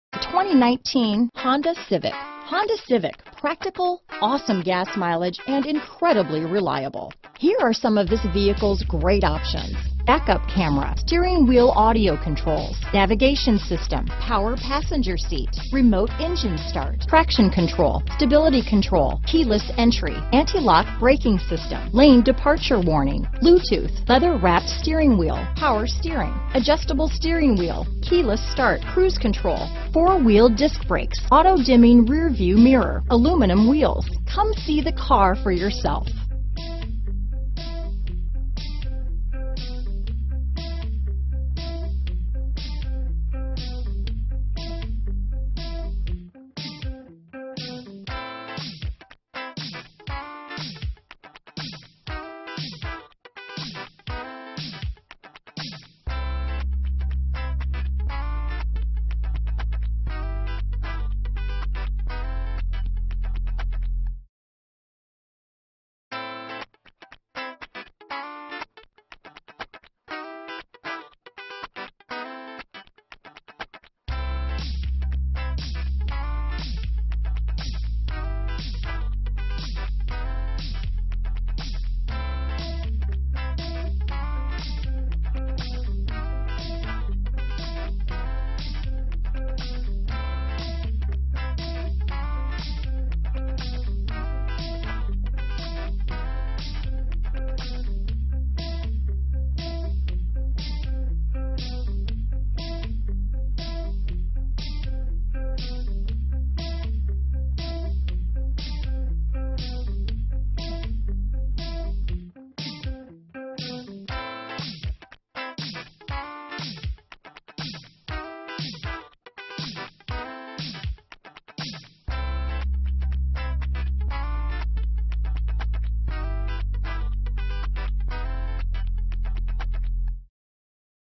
2019 Honda Civic Sedan Weatherford, Fort Worth, Arlington, Grapevine, Flower Mound H19261
Modern Steel Metallic New 2019 Honda Civic Sedan available in Weatherford, Texas at Honda of Weatherford. Servicing the Fort Worth, Arlington, Grapevine, Flower Mound, TX area.
2019 Honda Civic Sedan Touring - Stock#: H19261 - VIN#: JHMFC1F97KX011616

Boasts 38 Highway MPG and 30 City MPG! This Honda Civic Sedan has a dependable Intercooled Turbo Regular Unleaded I-4 1.5 L/91 engine powering this Variable transmission.* This Honda Civic Sedan is a Bargain with These Options *Wheels: 18 Alloy w/Gloss Shark Gray Inserts, VSA Electronic Stability Control (ESC), Valet Function, Trunk Rear Cargo Access, Trip Computer, Transmission: Continuously Variable (CVT) -inc: paddle shifters, Transmission w/Driver Selectable Mode, Tires: 235/40R18 91W AS, Systems Monitor, Strut Front Suspension w/Coil Springs.* Visit Us Today *Come in for a quick visit at Honda of Weatherford, 851 W I-20, Weatherford, TX 76087 to claim your Honda Civic Sedan!